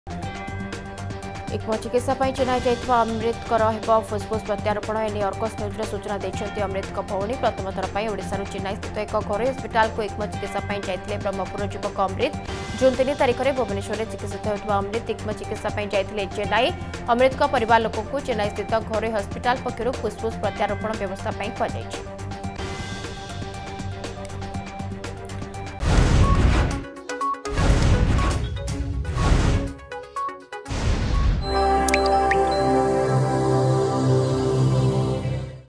ଅମ୍ରିତଙ୍କର ହେବ ଫୁସଫୁସ ପ୍ରତ୍ୟାରୋପଣ
ଇକମୋ ଚିକିତ୍ସା ପାଇଁ ଚେନ୍ନାଇ ଯାଇଥିବା ଅମ୍ରିତଙ୍କର ହେବ ଫୁସଫୁସ ପ୍ରତ୍ୟାରୋପଣ । ଏନେଇ ଅର୍ଗସ ନ୍ୟୁଜକୁ ସୂଚନା ଦେଇଛନ୍ତି ଅମ୍ରିତଙ୍କ ଭଉଣୀ ।  ପ୍ରଥମ ଥର ପାଇଁ ଓଡ଼ିଶାରୁ ଚେନ୍ନାଇ ସ୍ଥିତ ଏକ ଘରୋଇ ହସ୍ପିଟାଲକୁ ଇକମୋ ଚିକିତ୍ସା ପାଇଁ ଯାଇଥିଲେ ବ୍ରହ୍ମପୁରର ଯୁବକ ଅମ୍ରିତ୍ ।